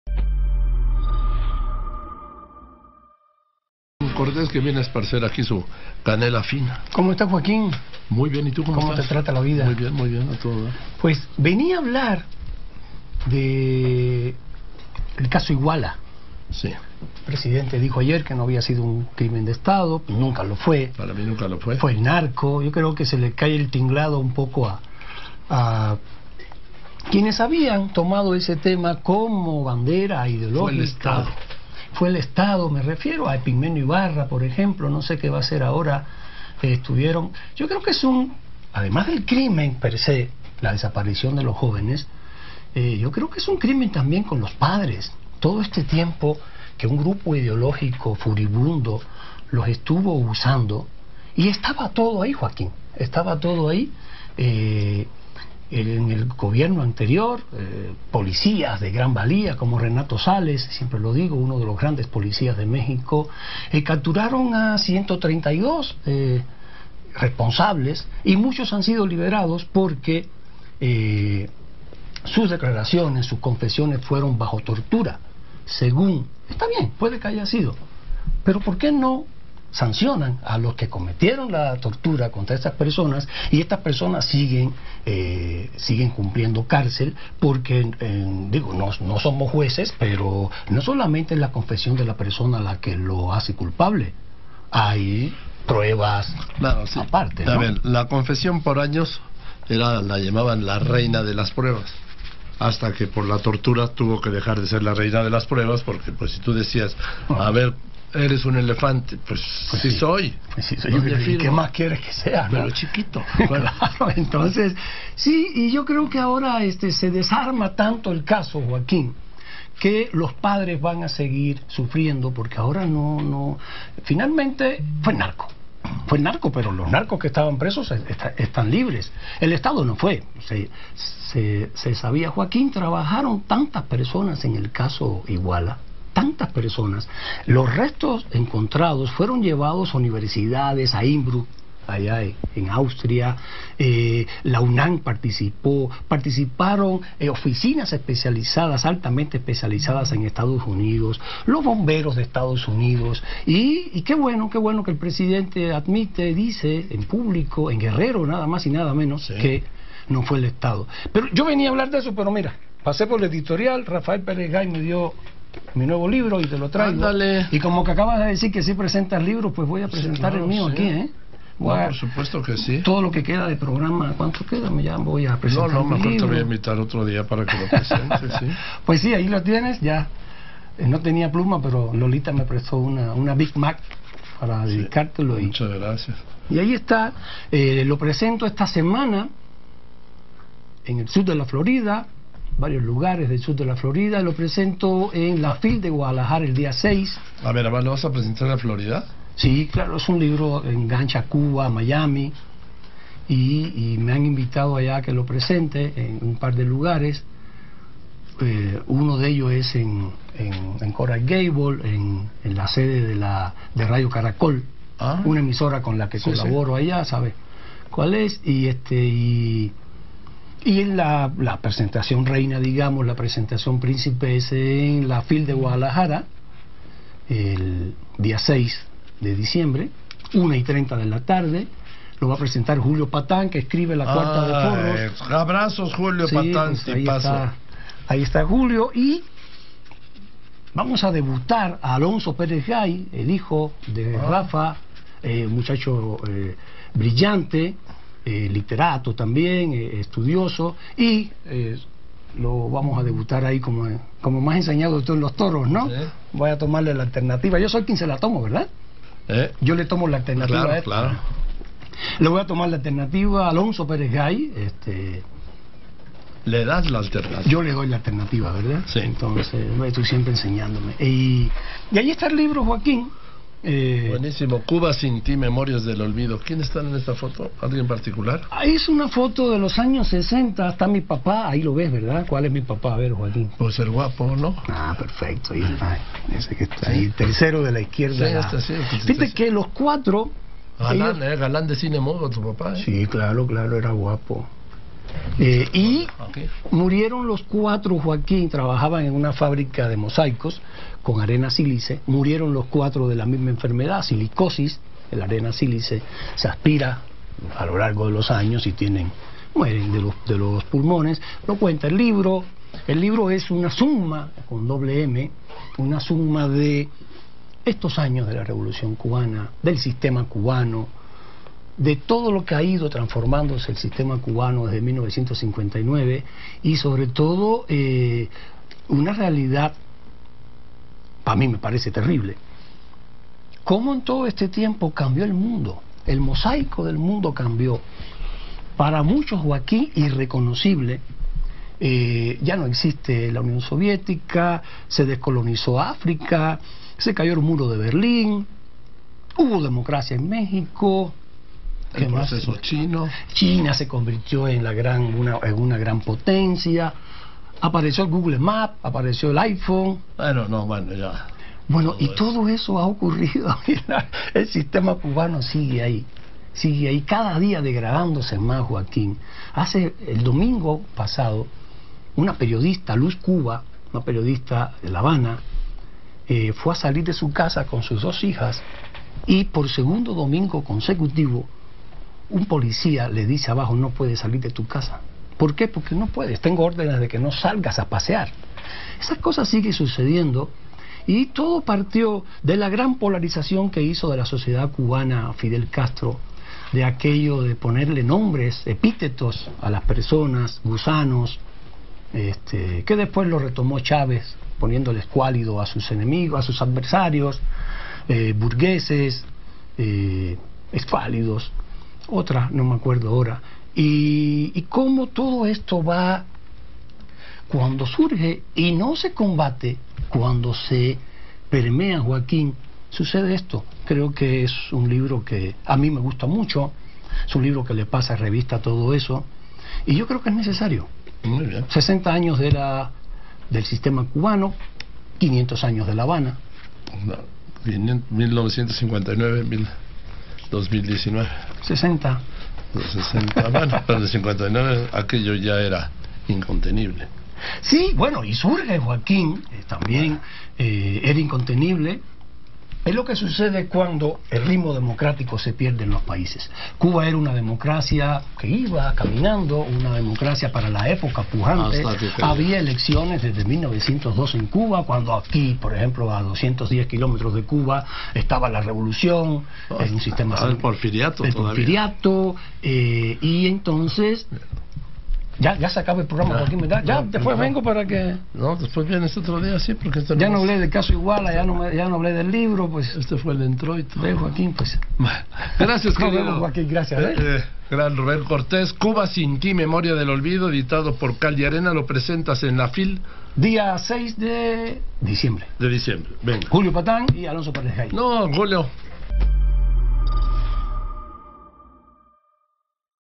Ayotzinapa ¿fue un crimen de Estado?
Rubén Cortés, columnista, analiza si realmente el Estado es responsable de lo ocurrido con los 43 estudiantes de Ayotzinapa